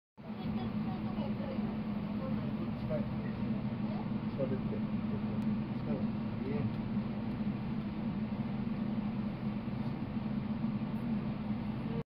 【車窓】〜大都会名古屋にキュートすぎる313系各駅停車２両編成〜313系特別快速から望む〜
車窓の他愛もない記録です